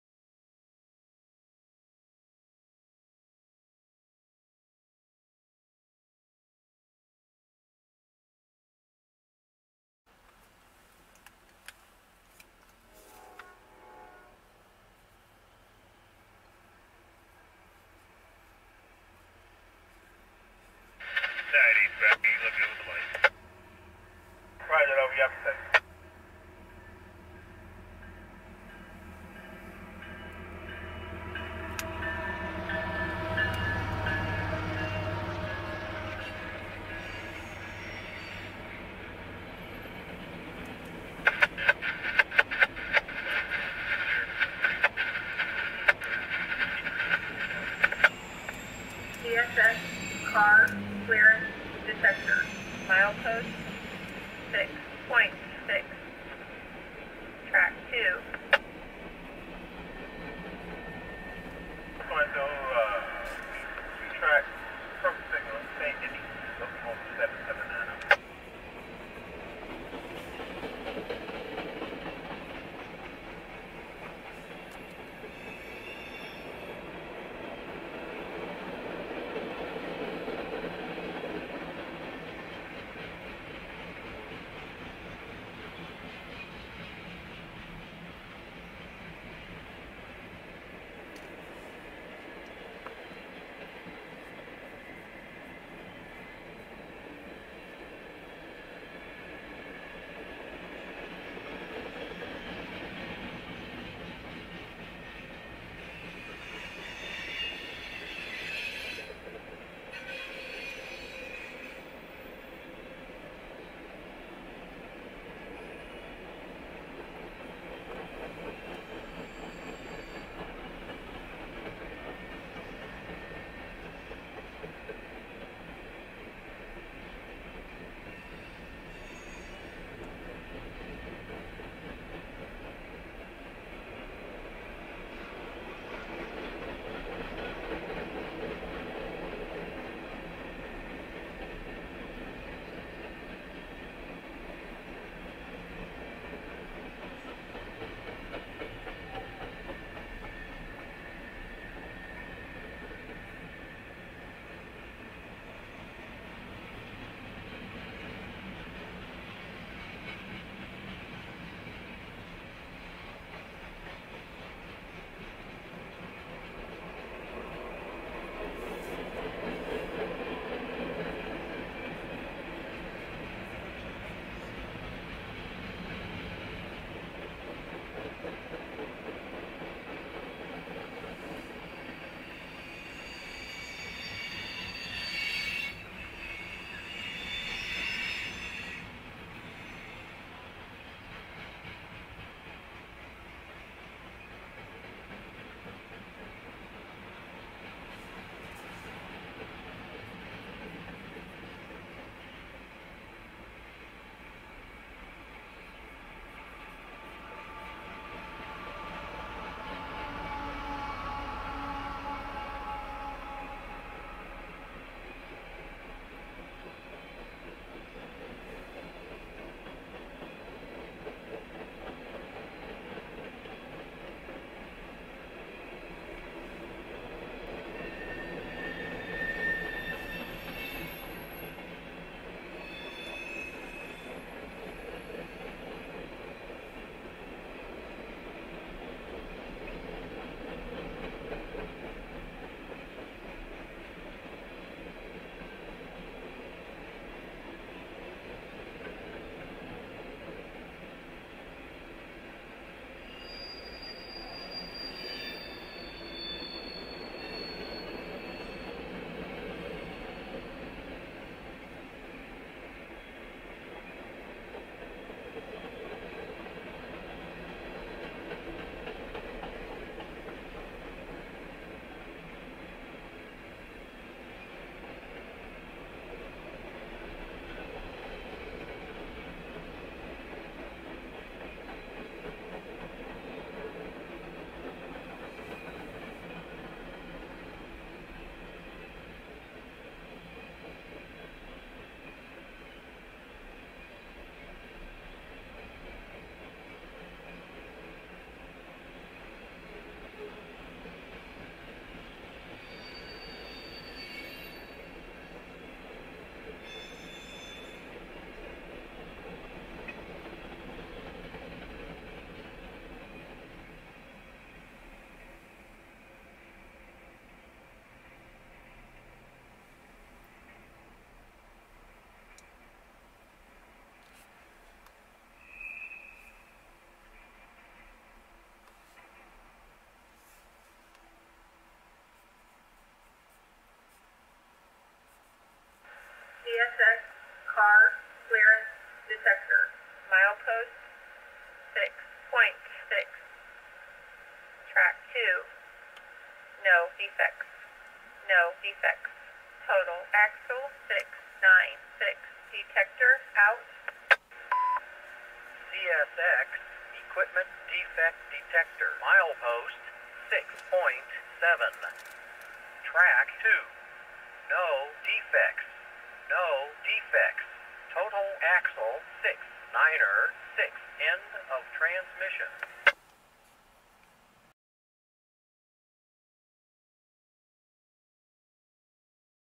M422
Two CSXT locomotives 779 (GE ES44AH YN3B) and mid train DPU 3412 (GE ET44AH YN3B) hauling M422 east bound on track two through Saint Denis Station. Car count 170 axle count 696. Filmed on 12/02/22 at 09:08.